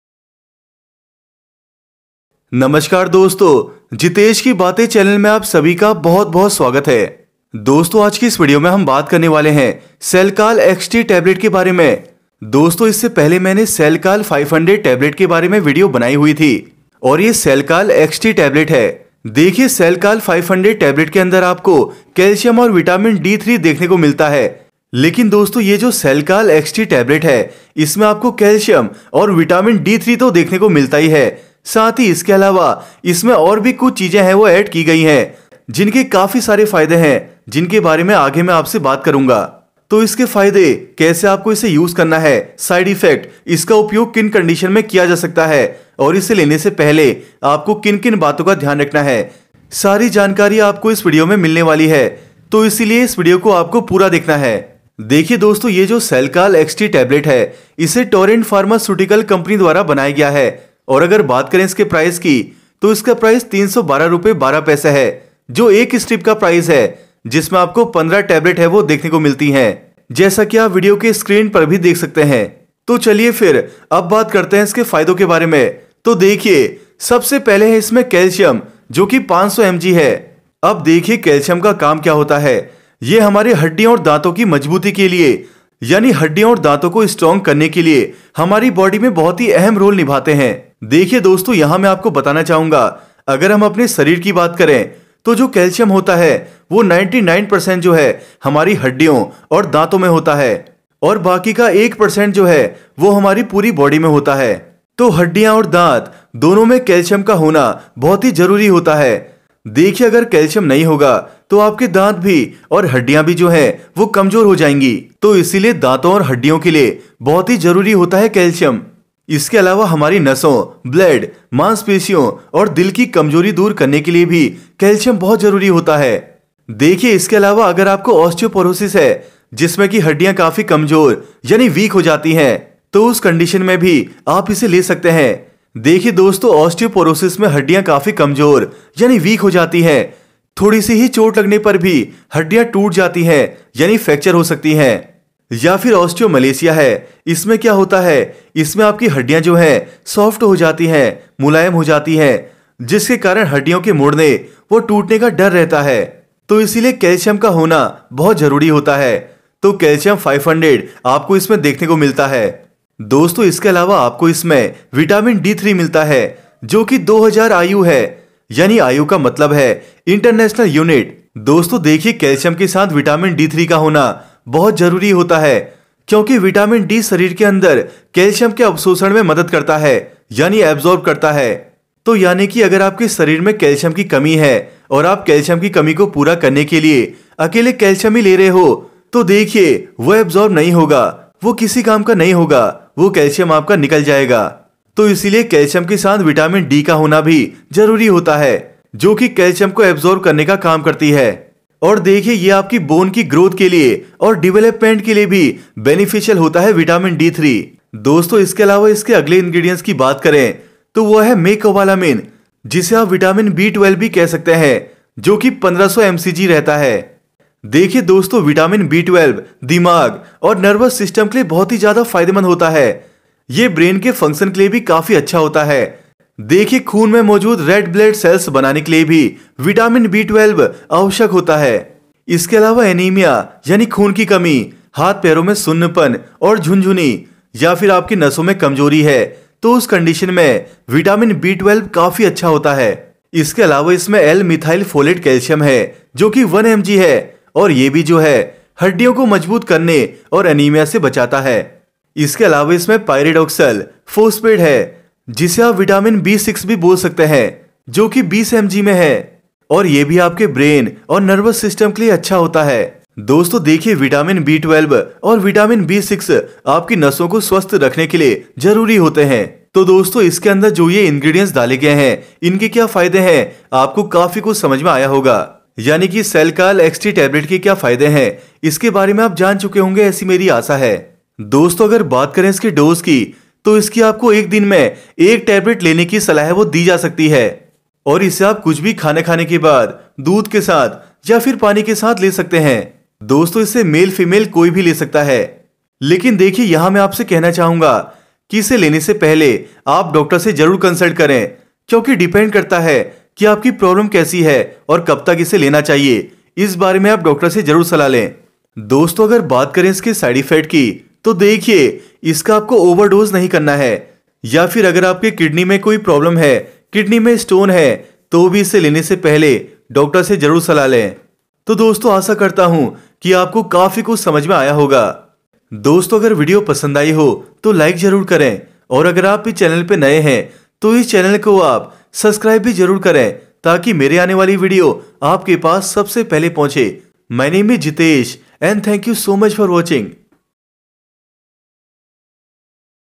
Shelcal XT Calcium And Vitamin Tablet Uses | Benefits | Dosage | Side Effects In Hindi
In this video I am sharing you information about shelcal xt
Useful links For You
My shampoo
Whey protein
Water bottle
Face Mask
My Gear:-
My Mic
My Tripod
My Mobile